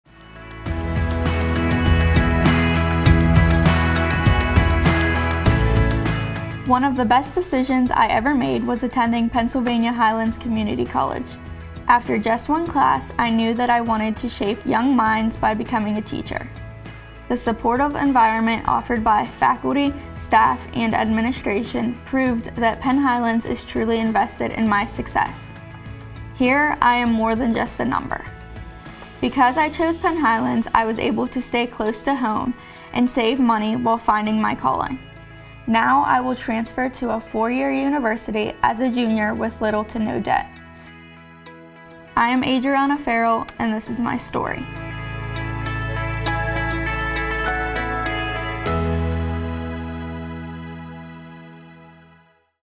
Adrianna Farrell's Story
"One of the best decisions I ever made was attending Pennsylvania Highlands Community College. After just one class, I knew that I wanted to shape young minds by becoming a teacher. The supportive environment offered by faculty, staff, and administration proved that Penn Highlands is truly invested in my success. Here, I am more than a number.

Because I chose Penn Highlands, I was able to stay close to home and save money while finding my calling. Now, I will transfer to a four-year university as a junior with little to no debt.

I’m Adrianna Farrell, and this is my story."

- Adrianna Farrell, Student Senate President & Student Trustee (March 2023)